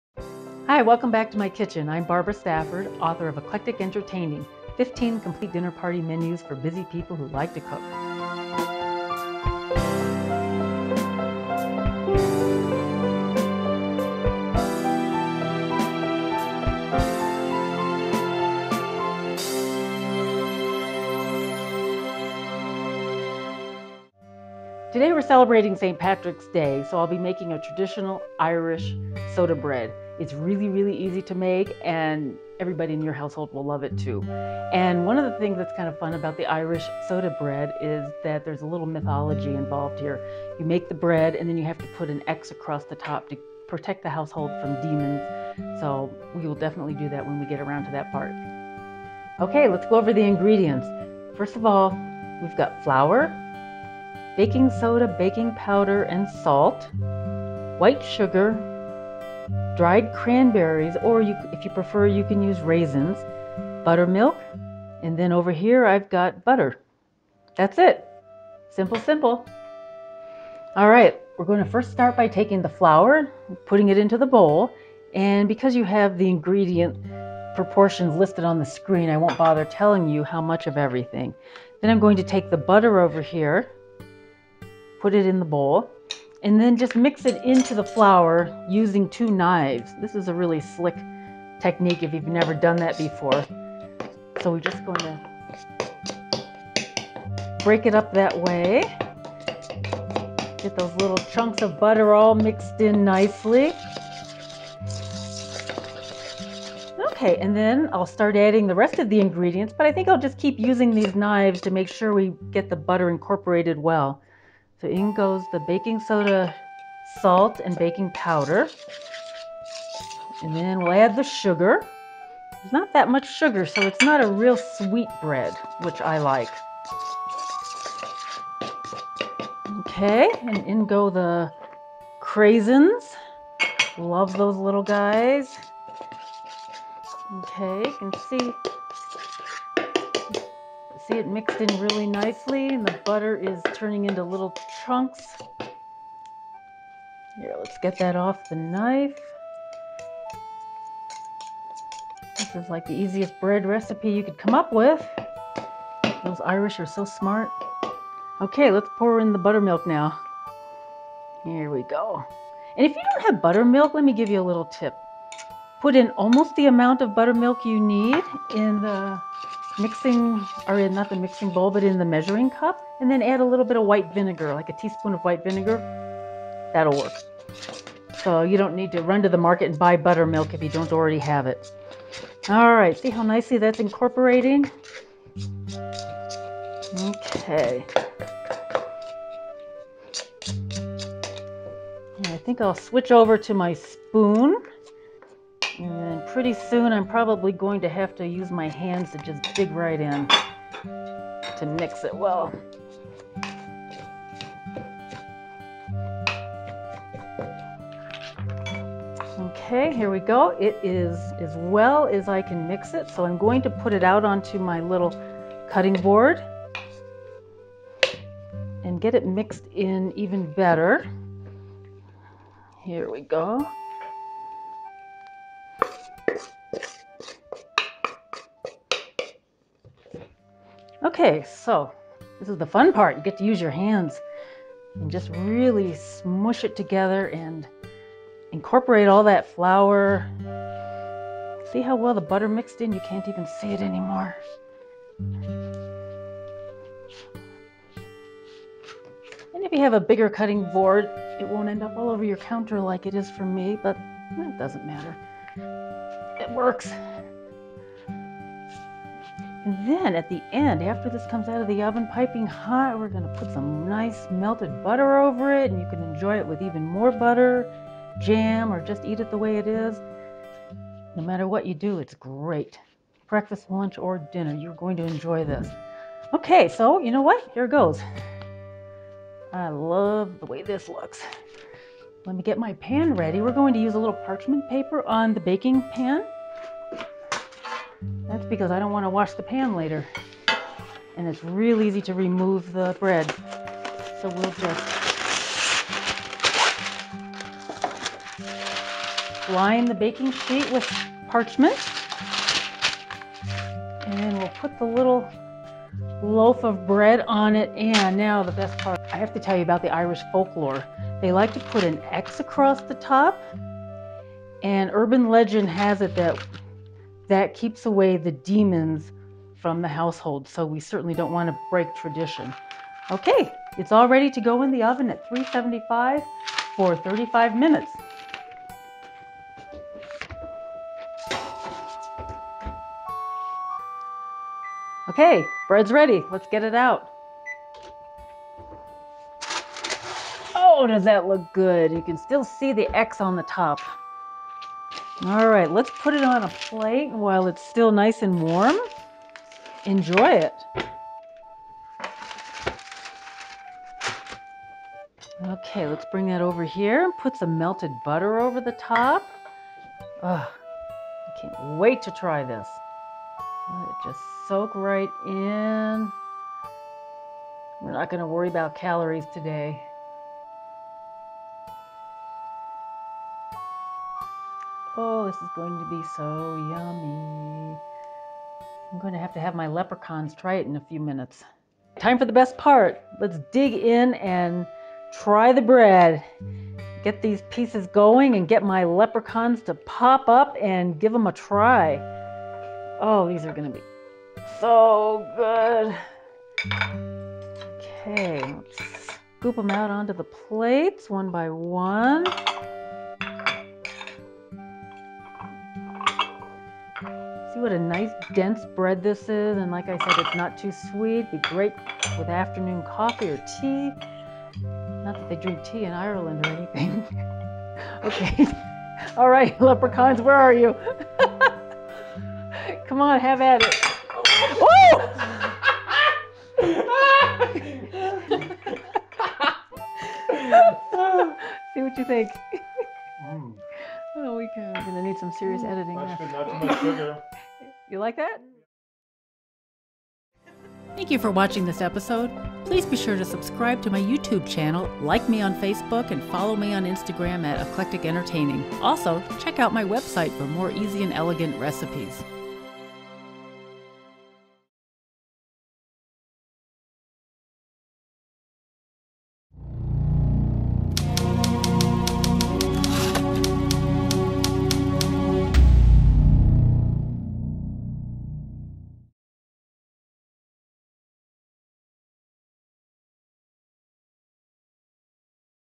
Eclectic Entertaining: St Patricks Day Traditional Irish Soda Bread
Celebrate St. Patrick's Day with this easy-to-prepare Traditional Irish Soda Bread recipe.

Follow Eclectic Entertaining for more video recipes and tutorials!